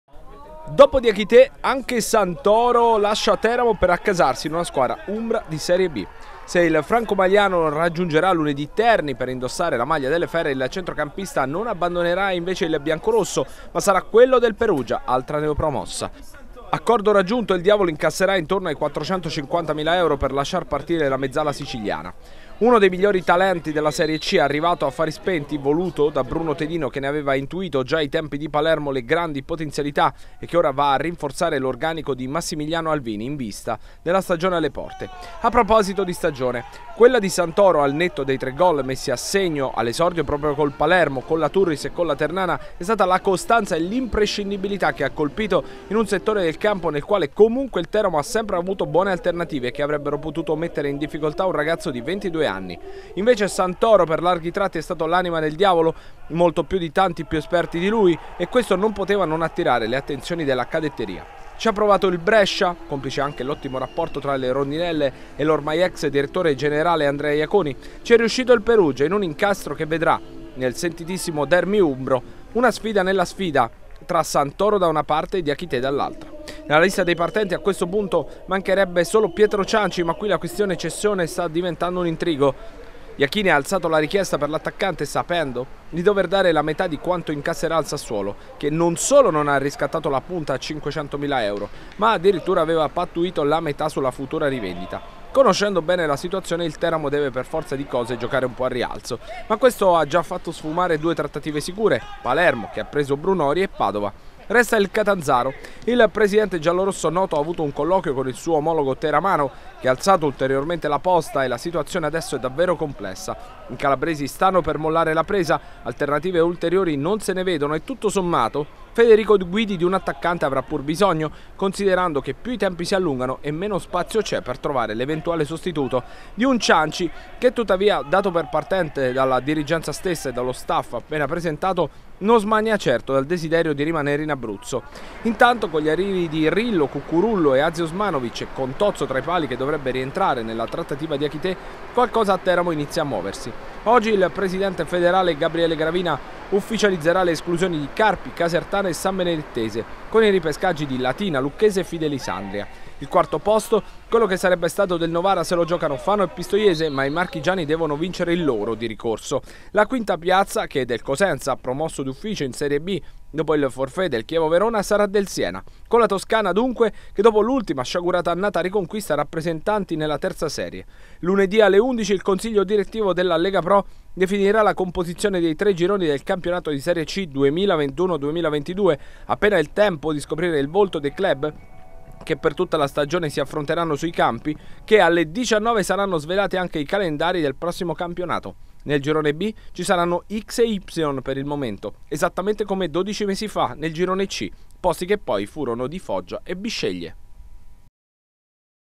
Teramo, Santoro va a Perugia. Lunedì gironi e calendari
Altra cessione per il Teramo. Simone Santoro va a Perugia in Serie B e, dopo Diakité, un altro protagonista della stagione passata sale di categoria. Resta il nodo Cianci ma, tra 48 ore, la Lega Pro, ufficializzerà gironi e calendari di terza serie